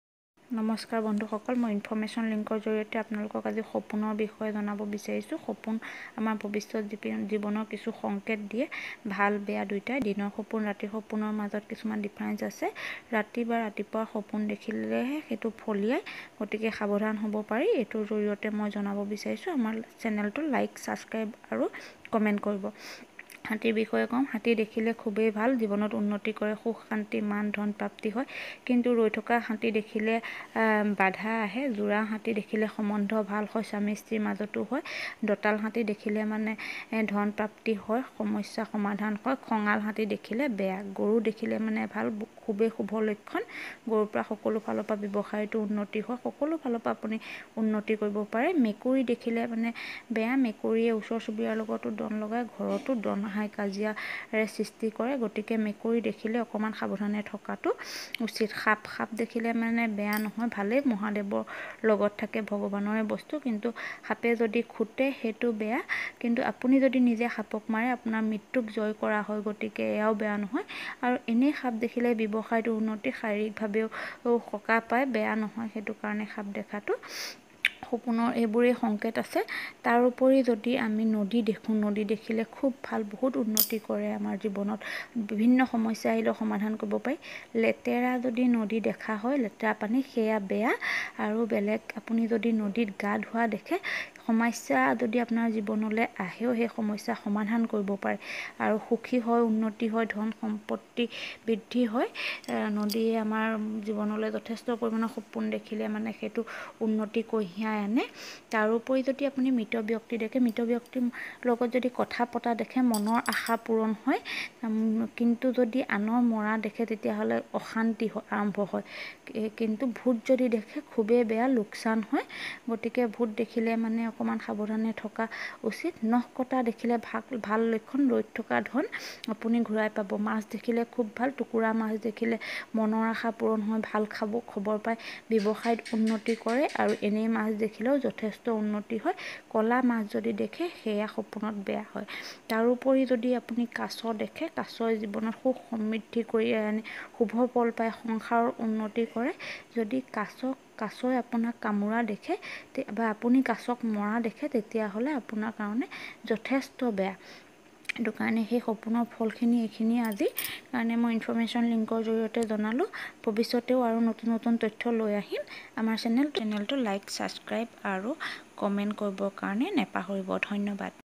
ৰাতি দেখা সপোনৰ ফল, শুভ অশুভ ফল
নদী,সাপ,হাতী,মৃত্যু,আদি সপোনত দেখিলে কি হব আপোনাৰ জীৱনত।